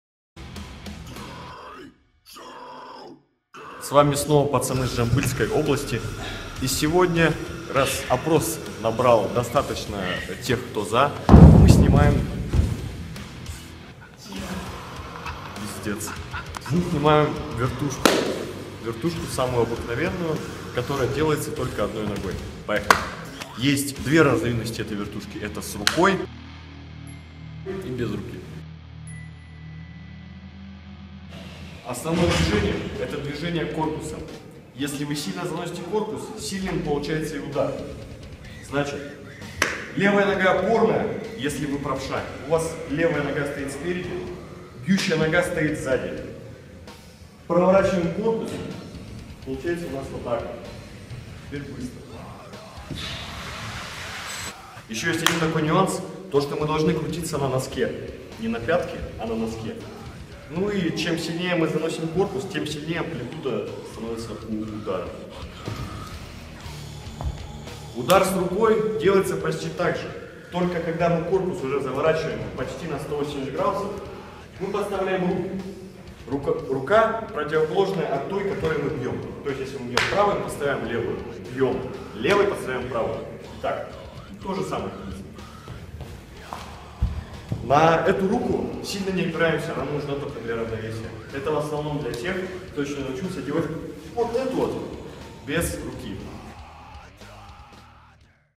Mosh Blog - 1 серия
В этой серии мы рассказываем, как делать удар с разворотом корпуса (вертуха обыкновенная)